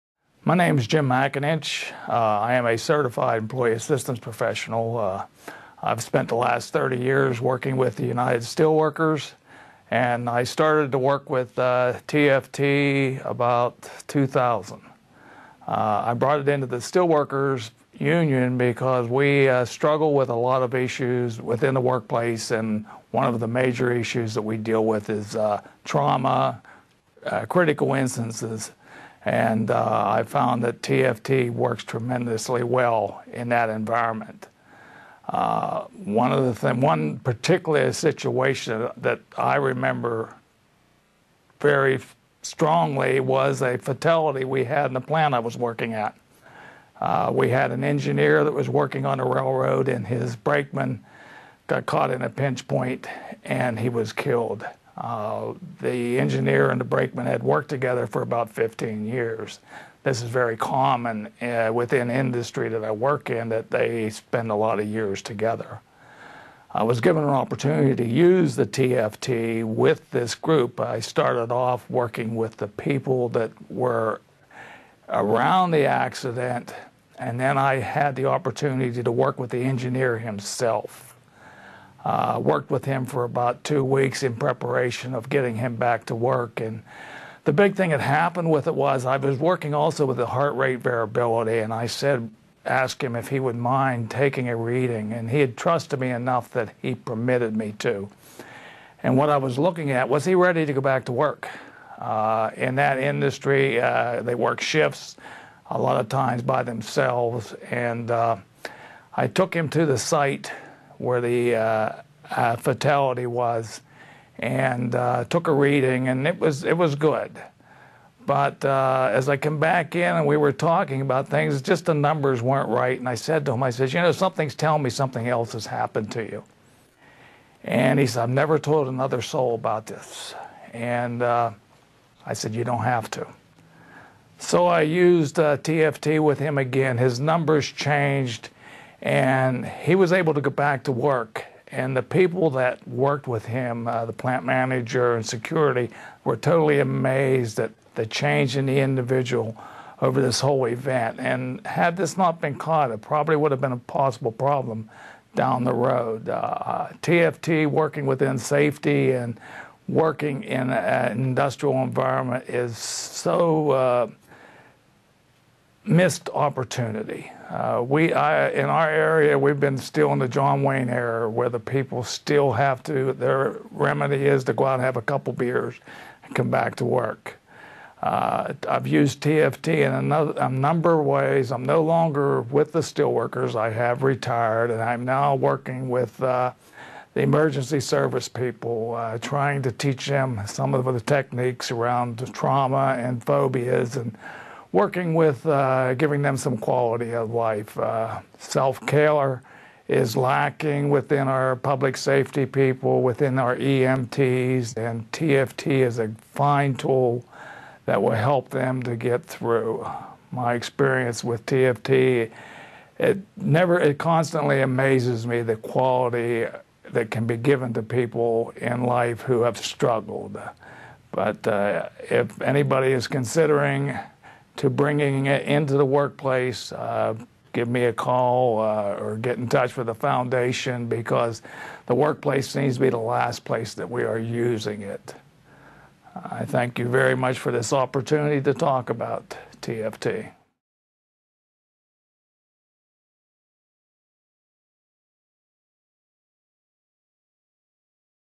Jim McAninch - TFT In The Workplace
Jim McAninch is a counselor with "Solutions to Stress, Anxiety & Toxins" in Tarentum, PA, and an Employee Assistance specialist for the United Steelworkers, Local 1138. He is a Certified Trauma Responder, a Certified Employee Assistance Professional, and a Certified TFT Practitioner (Diagnosis Level).